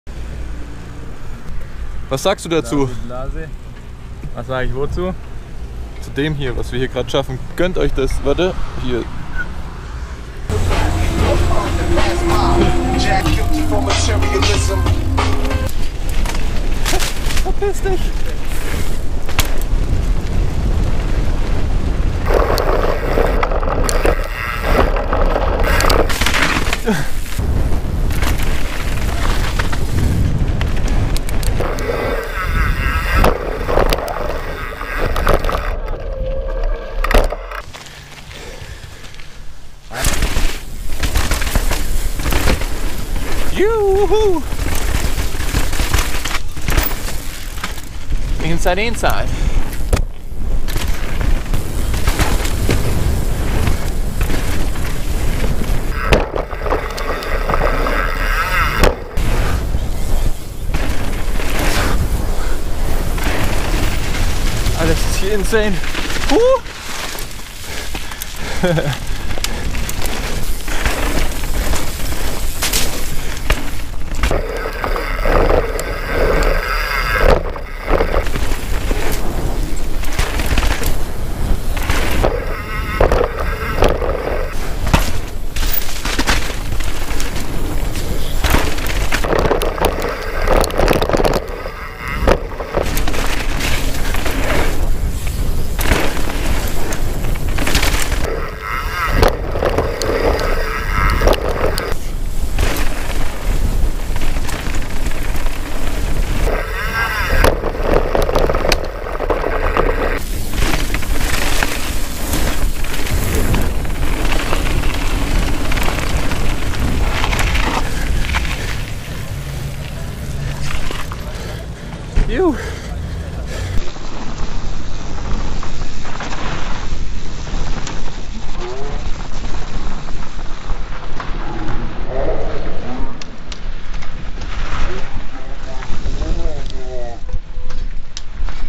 Lac Blanc - La Fat from another Point of View | Synergy Media
Am letzten Wochenenden haben wir für euch mal neue, frische Perspektiven getestet! Ein Run auf der La Fat aus einer etwas anderen Sichtweise. Wie gefällt es euch? Gefilmt mit der GoPro Hero 4 Black. Schreibt in die Kommentare, wie es euch gefällt und ob wir ein Video zu den Halterungen machen sollen?